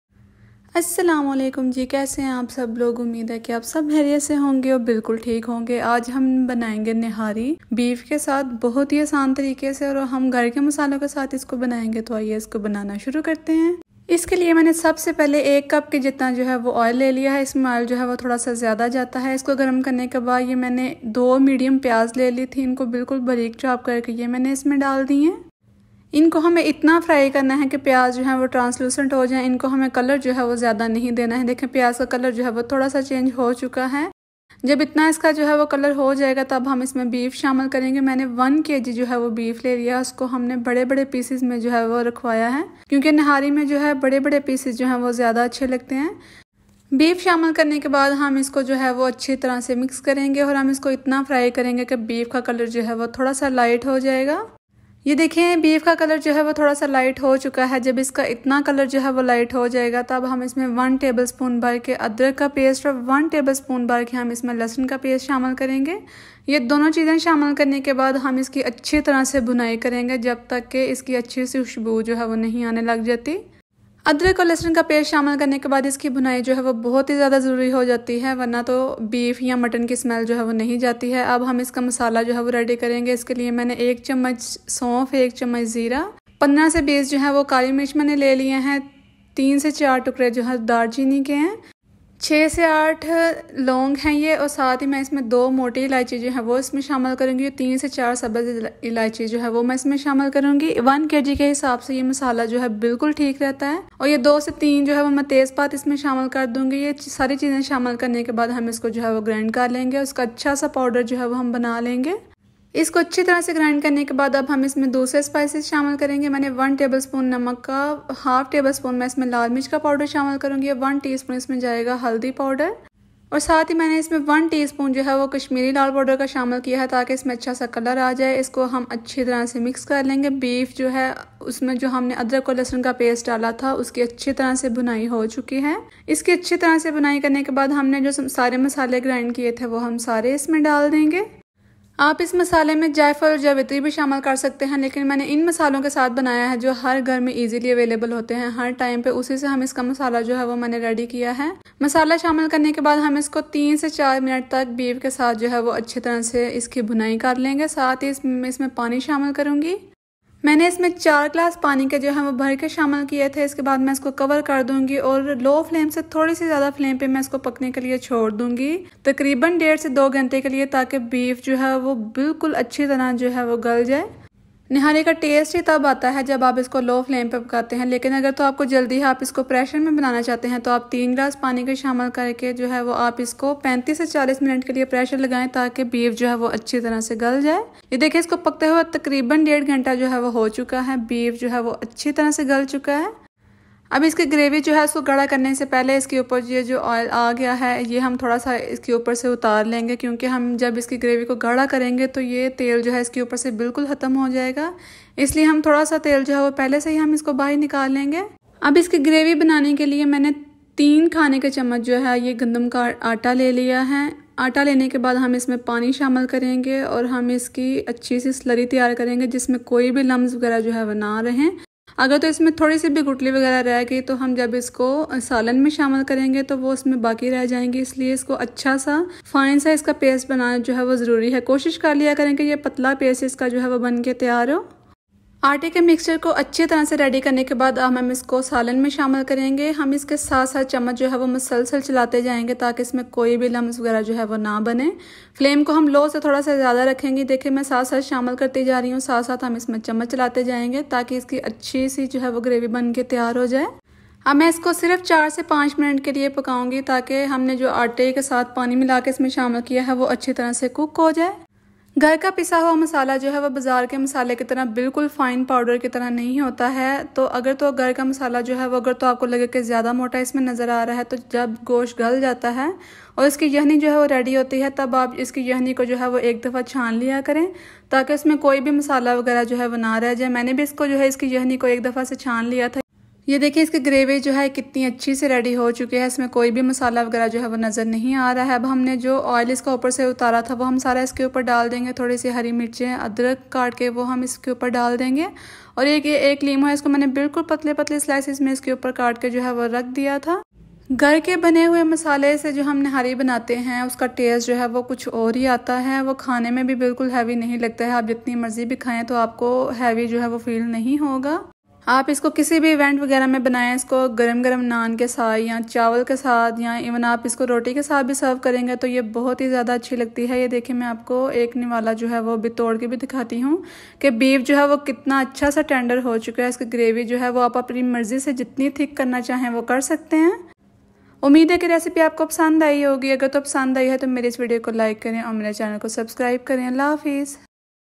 Beef Nihari Recipe By Kitchen With Farah ||How To Make Nihari With Homemade Masala
Beef Nihari

Beef Nihari is a traditional Pakistani and Indian dish known for its rich and flavorful taste. It's a slow-cooked stew made with tender beef, spices, and a mixture of aromatic herbs. The dish is usually prepared overnight or for several hours, allowing the flavors to meld and the meat to become incredibly tender. Nihari is often served as a hearty breakfast or special meal, accompanied by naan (Indian flatbread) or rice. The thick, flavorful gravy is the highlight of the dish, and it's often garnished with fresh ginger, chopped cilantro, green chilies, and fried onions before serving.
Ingredients:
Beef-1 kg
Oil-1 cup

Nihari masala
Zeera(cumin)-1 tbsp
Sonf(fennel)-1 tbsp
Black pepper-15,20
Laung(cloves)8,10
Dar cheeni(cinnamon)2,3 small pieces
Taiz paat(bayleaf)-2,3
Salt-1 tbsp
Red pepper-1/2 tbsp
Turmeric-1 tsp
Kashmiri Laal powder (optional)1 tsp

Wheat flour-3,4 tbsp

For garnishing:
Chopped Green chillies
Chopped ginger
Lemon slices

How to make Nihari?
Nihari bnany ka Tarika?
Nihari banany ka Tarika?
How to make beef Nihari?
How to make mutton Nihari?